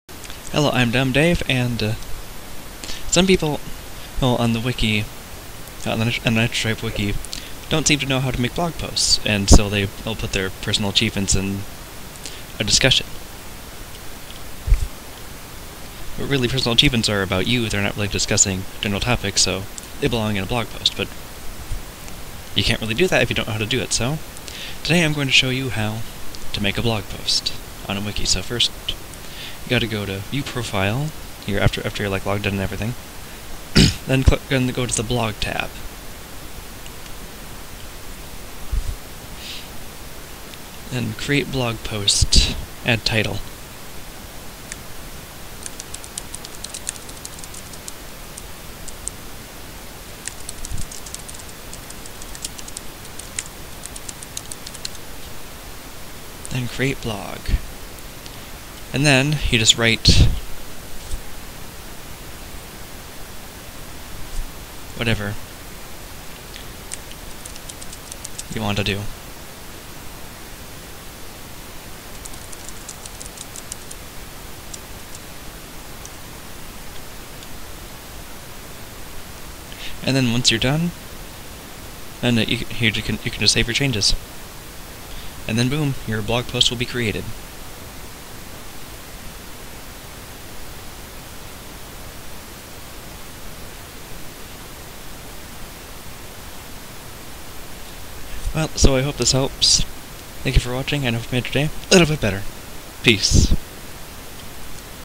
How to Make a Blog Post on Fandom Wikis
Tutorial that teaches how to create blog posts on wikis. and I will do my best to answer them.